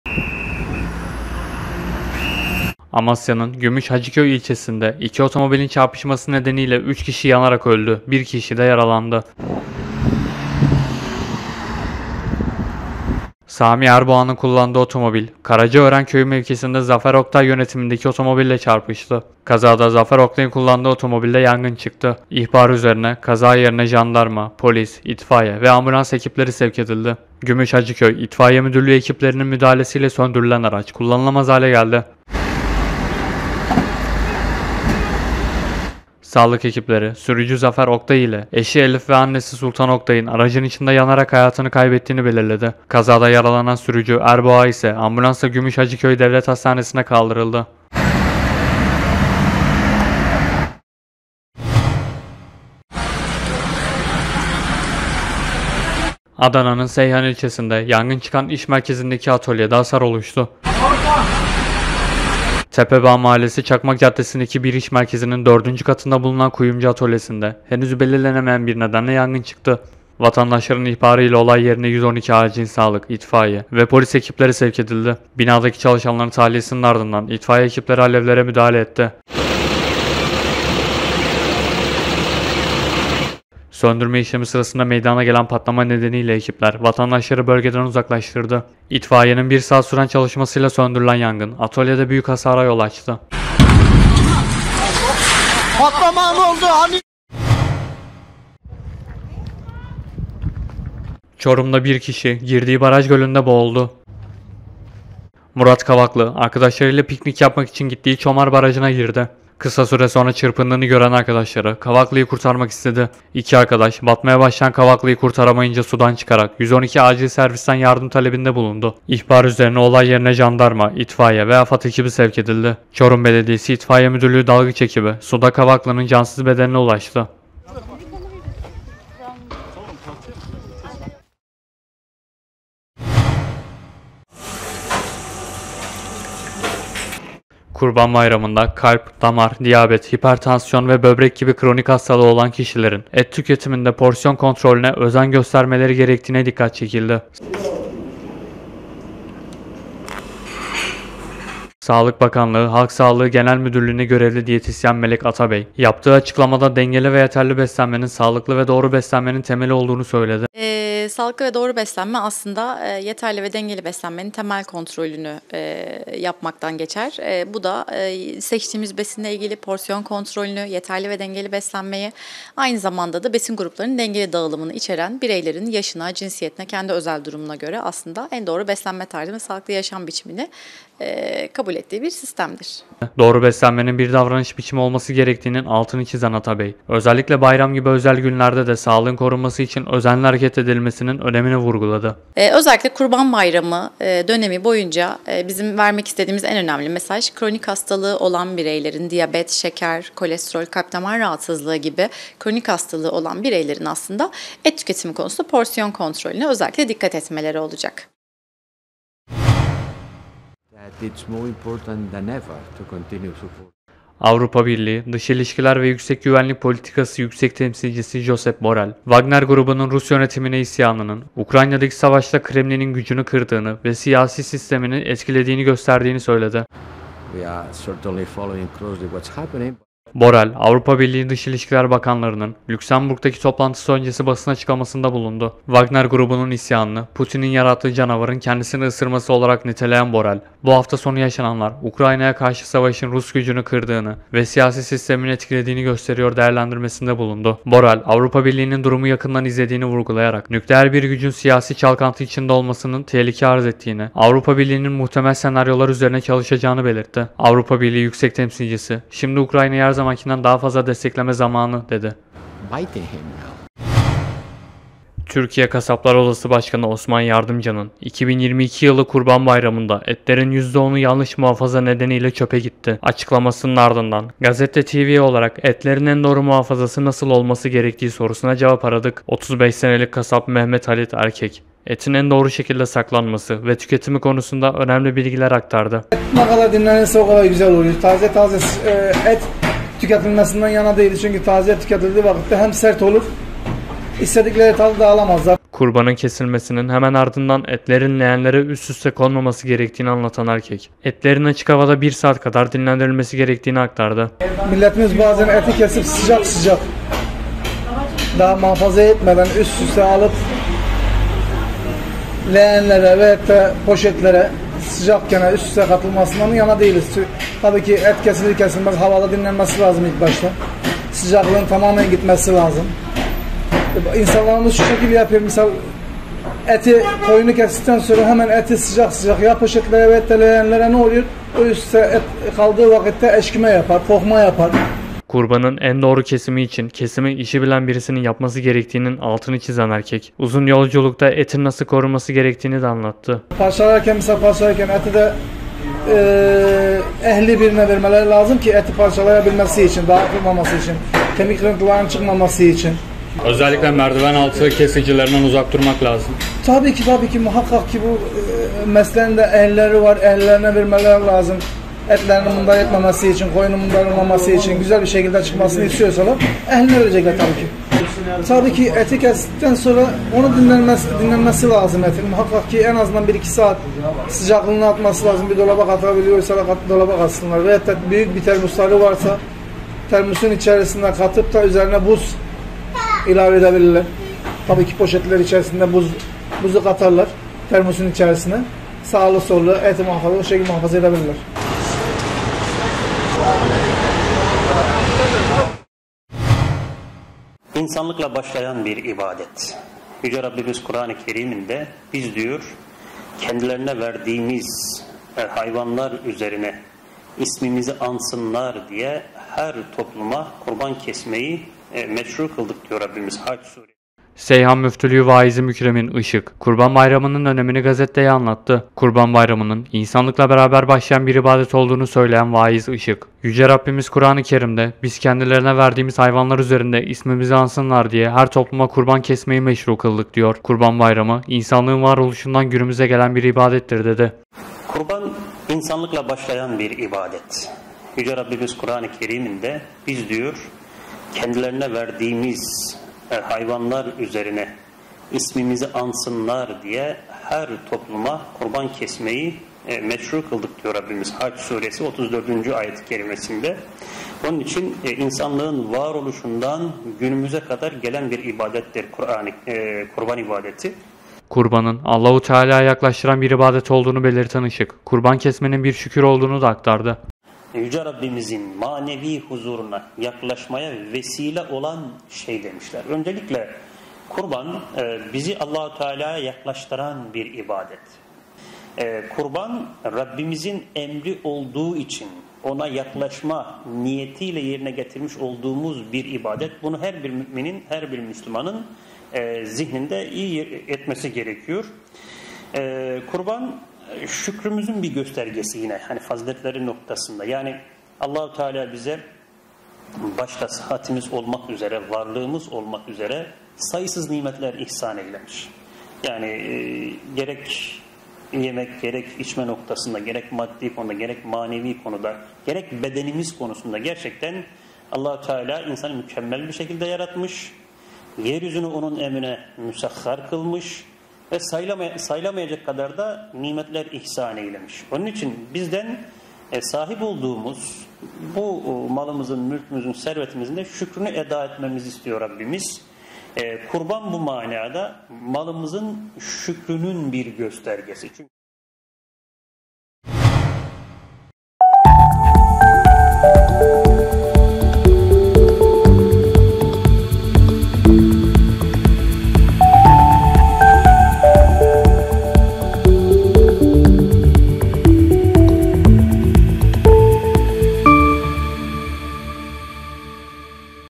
3 KİŞİ YANARAK CAN VERDİ - 26 HAZİRAN 2023 HABER BÜLTENİ
Amasya’nın Gümüşhacıköy ilçesinde iki otomobilin çarpışması nedeniyle 3 kişi yanarak öldü, bir kişi yaralandı.